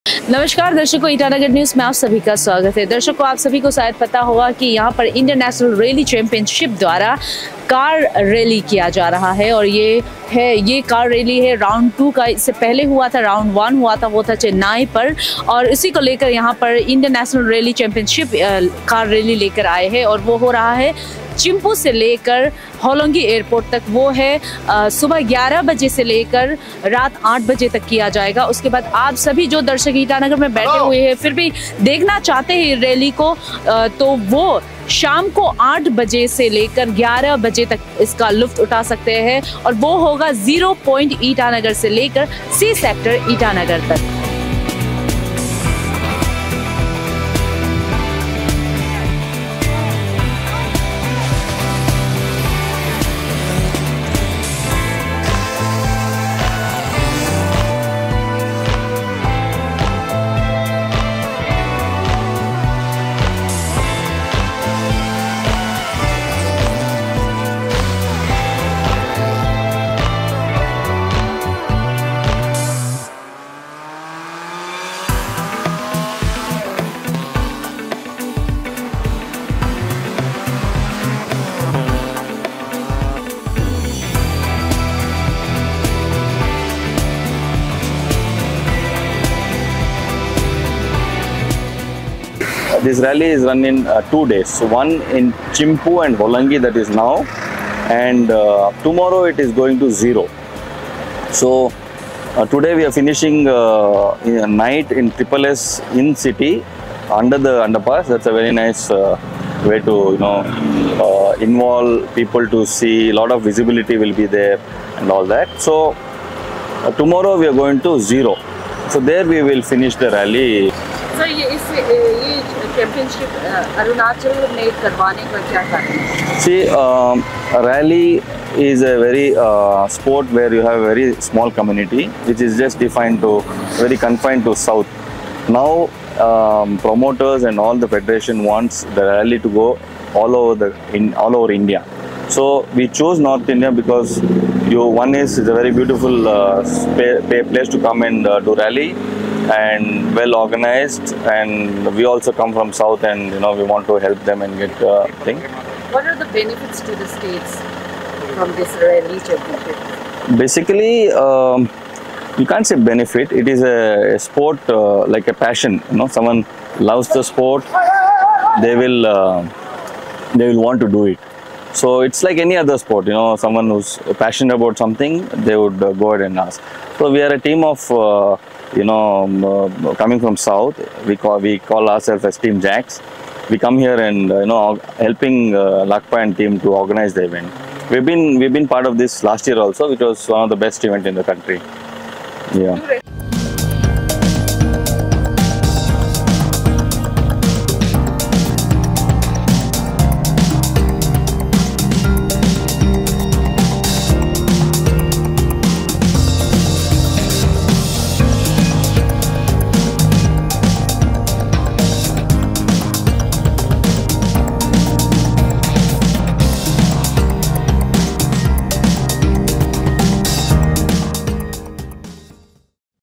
Thrilling starts to the Rally of Arunachal 2023 as participants kick off from Chimpu to Hollongi.
"Thrilling start to The Rally of Arunachal 2023 as participants kick off from Chimpu to Hollongi, with Yazali to Ziro route up next!"